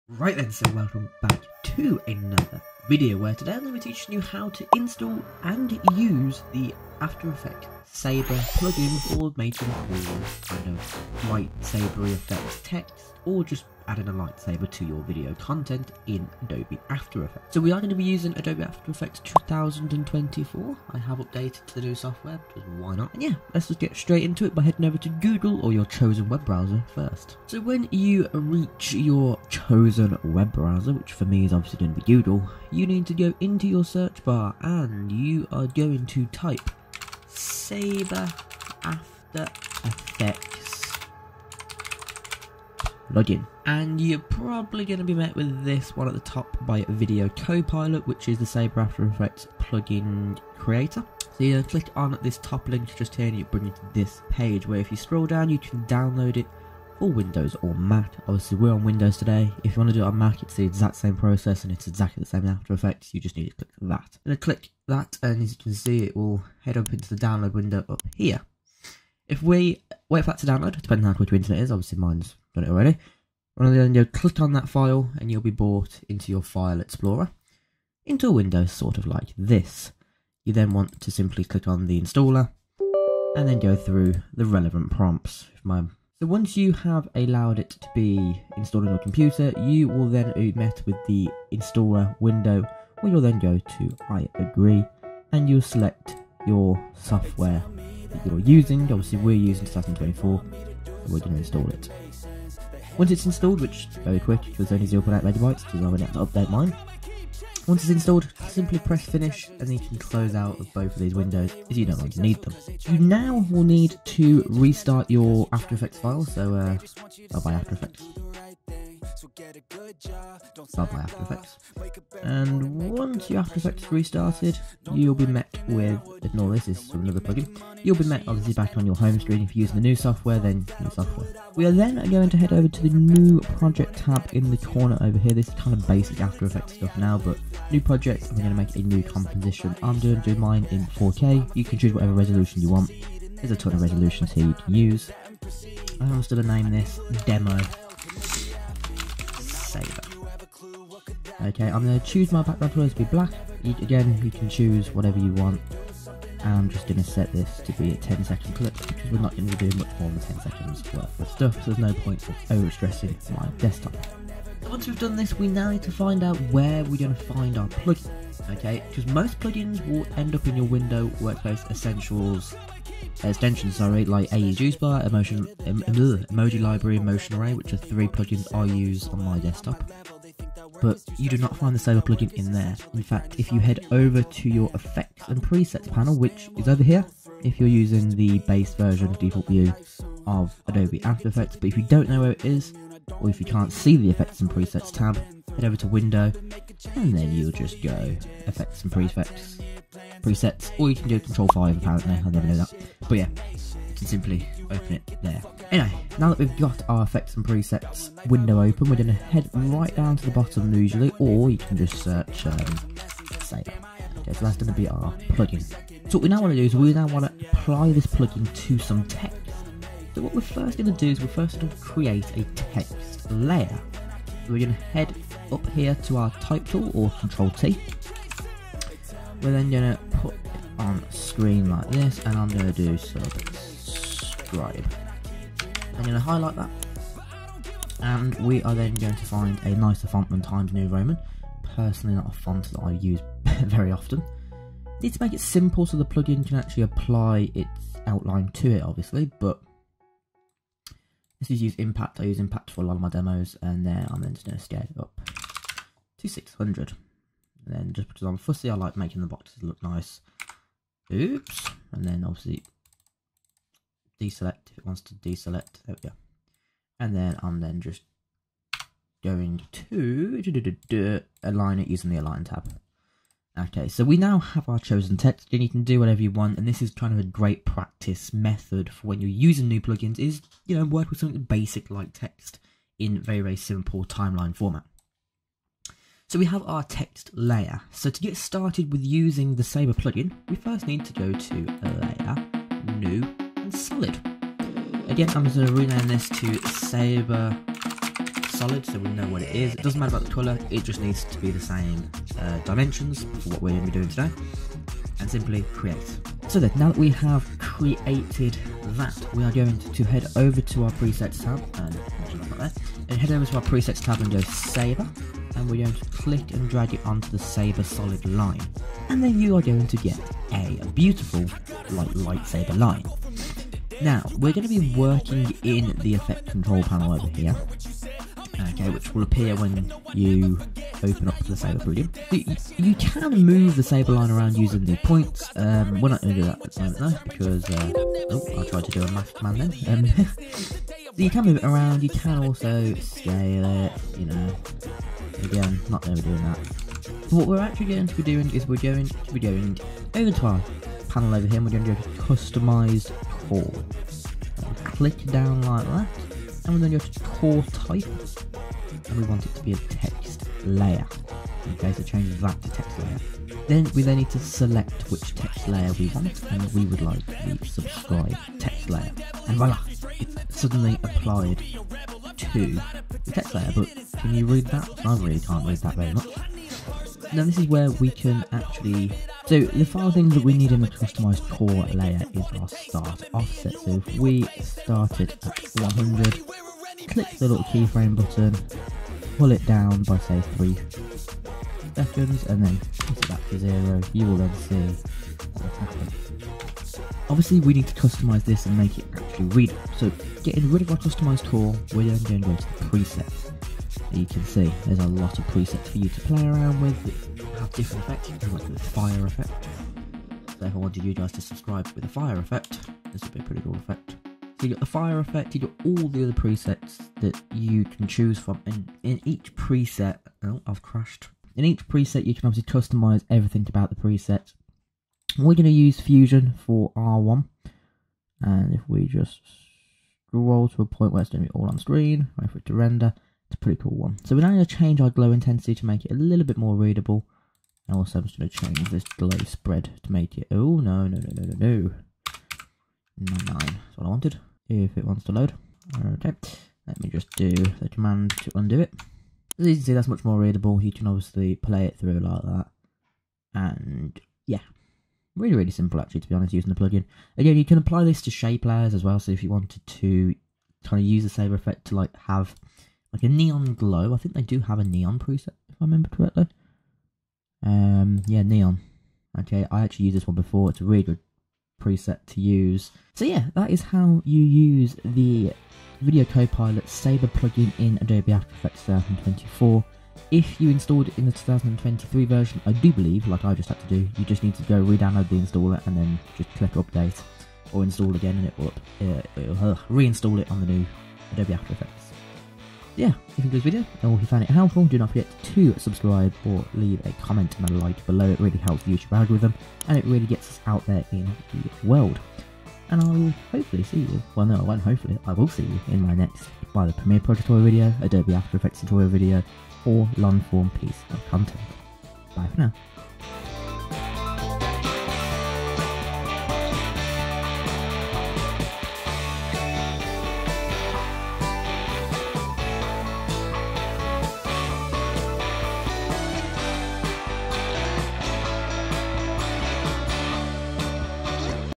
Saber Plugin Tutorial - Adobe After Effects 2024
Ever wanted to level up your videos with a simple addition - like a lightsaber? Today I'll show you how to install and use the Saber plugin for Adobe After Effects!

✍️ Are you an editor that needs some high-quality, easy-to-use assets in your next video? AEJuice has you covered